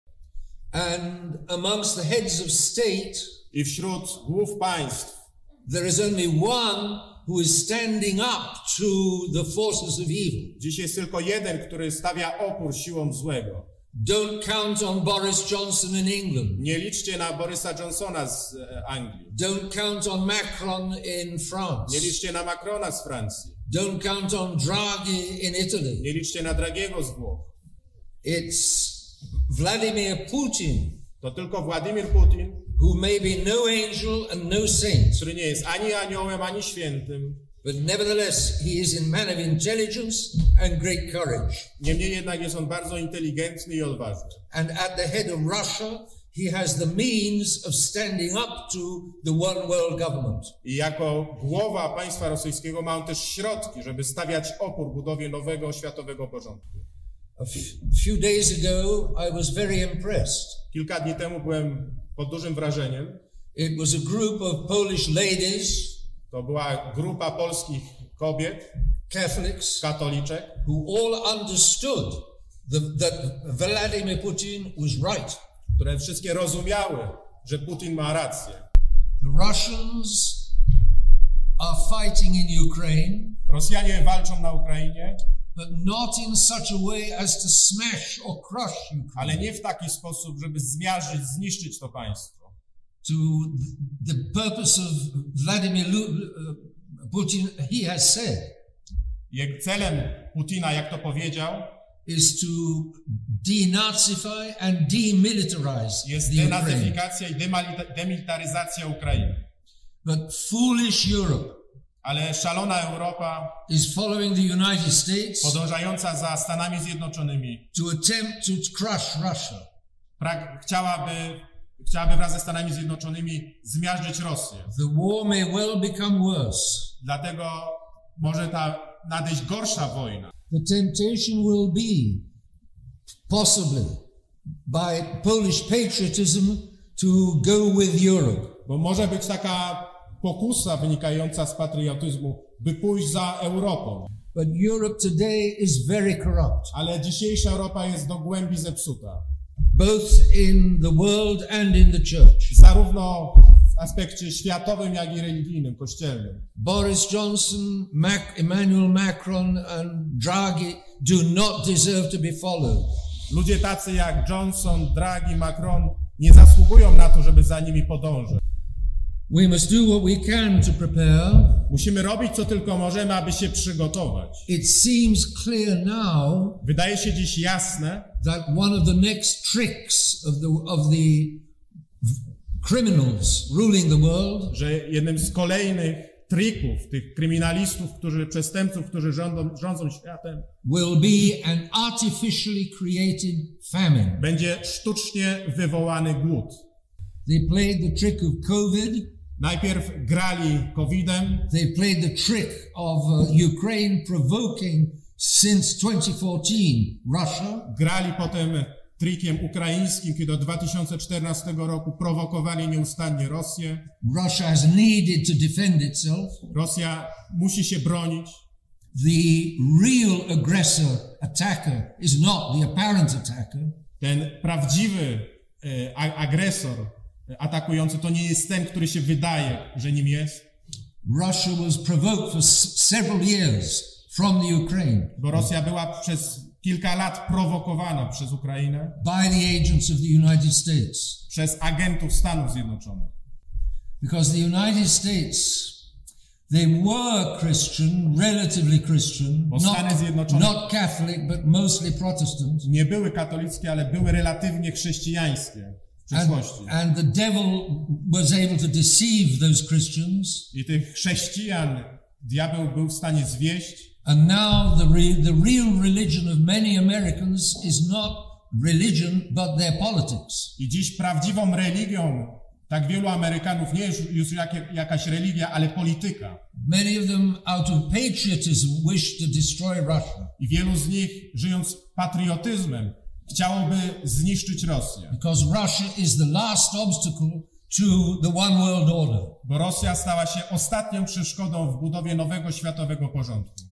Bishop Richard Williamson on Ukraine War
British Bishop Richard Williamson on Ukraine War.
Angielski biskup o sytuacji na Ukrainie.
أسقف إنجليزي يتحدث عن الوضع في أوكرانيا